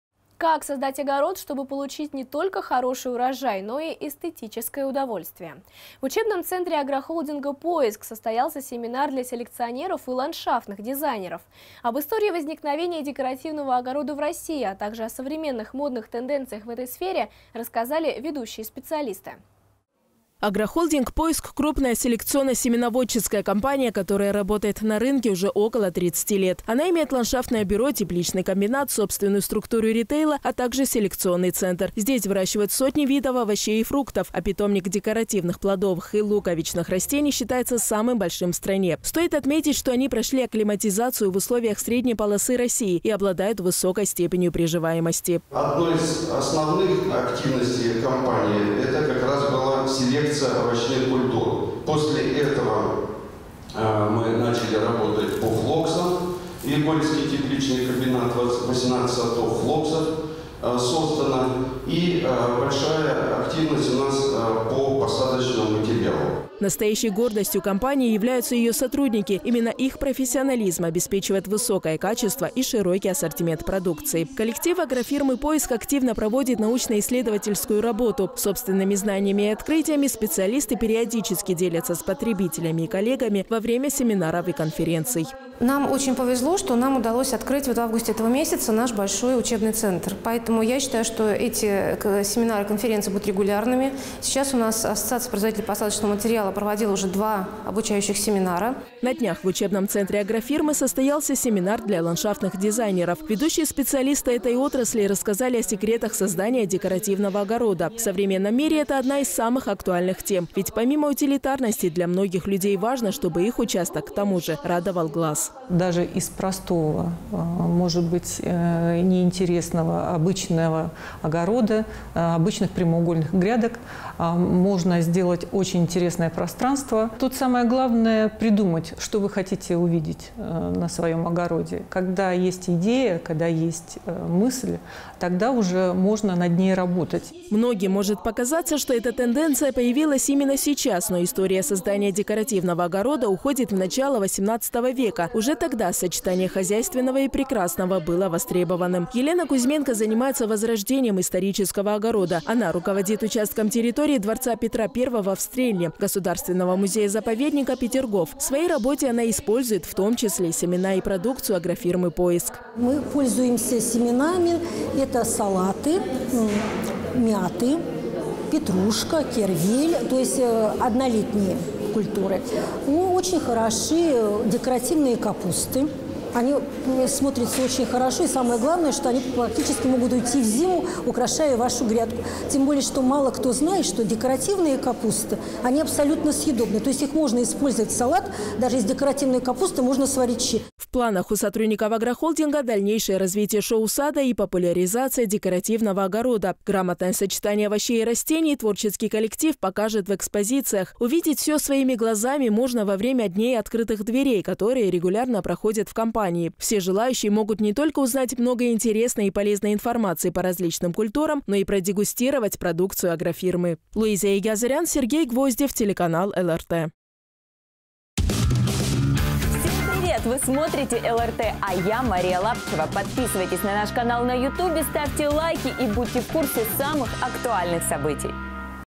Агрофирма «Поиск» провела семинар для ландшафтных дизайнеров
Как создать огород, чтобы получить от него не только хороший урожай, но и эстетическое удовольствие? В учебном центре агрохолдинга «Поиск» состоялся семинар для селекционеров и ландшафтных дизайнеров. Об истории возникновения декоративного огорода в России, а также о современных модных тенденциях им рассказали ведущие специалисты.

Агрохолдинг «Поиск» - крупная селекционно-семеноводческая компания, которая работает на рынке уже около 30 лет. Она имеет ландшафтное бюро,  тепличный комбинат, собственную структуру ритейла,  а также селекционный центр. Здесь выращивают сотни видов овощей и фруктов, а питомник декоративных, плодовых и луковичных растений считается самым большим в стране. Стоит отметить, что они прошли акклиматизацию в условиях средней полосы России и обладают высокой степенью приживаемости.

Сергей Максимов, член Совета директоров агрохолдинга «ПОИСК»:
"Одной из основных активностей компании это, как раз, была, селекция овощных культур. После этого мы начали работать по флоксам, Егорьевский тепличный комбинат. 18 сортов флоксов создано, и большая активность у нас по посадочному материалу".

Настоящей гордостью компании являются ее сотрудники, именно их профессионализм обеспечивает высокое качество и широкий ассортимент продукции. Коллектив агрофирмы «ПОИСК» активно проводит научно-исследовательскую работу. Собственными знаниями и открытиями специалисты периодически делятся с потребителями и коллегами во время семинаров и конференций.

Оксана Пискарева, руководитель Ландшафтного бюро «GARDIE» агрохолдинга «ПОИСК»:
"Нам очень повезло, что нам удалось открыть в августе наш большой учебный центр, поэтому, я считаю, что эти семинары, конференции будут регулярными. Ассоциация производителей посадочного материала провела уже 2 обучающих семинара".

На днях в учебном центре агрохолдинга состоялся семинар для ландшафтных дизайнеров. Ведущие специалисты этой отрасли рассказали о секретах создания декоративного огорода. В современном мире – это одна из самых актуальных тем, ведь помимо утилитарности для многих людей  важно, чтобы их участок, к тому же, радовал глаз.

Ирина Сахарова, ландшафтный дизайнер:
"Даже из простого, неинтересного, обычного огорода, обычных прямоугольных грядок можно сделать очень интересное пространство. Тут самое главное- придумать, что вы хотите увидеть на своем огороде, когда есть идея, когда есть мысль, тогда уже можно над ней работать".

Многим может показаться, что эта тенденция появилась именно сейчас, но история создания декоративного огорода уходит в начало 18 века. Уже тогда сочетание хозяйственного и прекрасного было востребованным. Елена Кузьменко занимается возрождением исторического огорода, она руководит участком территории Дворца Петра  1 в Стрельне, государственного музея – заповедника «Петергоф». В своей работе она использует, в том числе, семена и продукцию агрофирмы «ПОИСК».

Елена Кузьменко, начальник участка территории Дворца Петра I в Стрельне Государственного музея-заповедника « Петергоф»:
"Мы пользуемся семенами – это салаты, мята, петрушка, кервель, то есть однолетние культуры. Очень хороши декоративные капусты, они смотрятся очень хорошо, и, самое главное, что они могут уйти в зиму, украшая вашу грядку. Мало кто знает, что декоративные капусты абсолютно съедобны, их можно использовать в салат, также из декоративной капусты можно сварить щи".

В планах у сотрудников агрохолдинга - дальнейшее развитие шоу-сада  и популяризация декоративного огорода. Грамотное сочетание овощей и растений творческий коллектив покажет в экспозициях. Увидеть все своими глазами можно во время дней открытых дверей, которые регулярно проходят в компании. Все желающие могут не только узнать много интересной и полезной информации по различным культурам, но и продегустировать продукцию агрофирмы.

Агрохолдинг «ПОИСК» - Московская область, Раменский район, дер. Верея, Островецкое шоссе, стр. 500,501
Интернет магазин online.semenasad.ru

Луиза Егиазарян. Сергей Гвоздев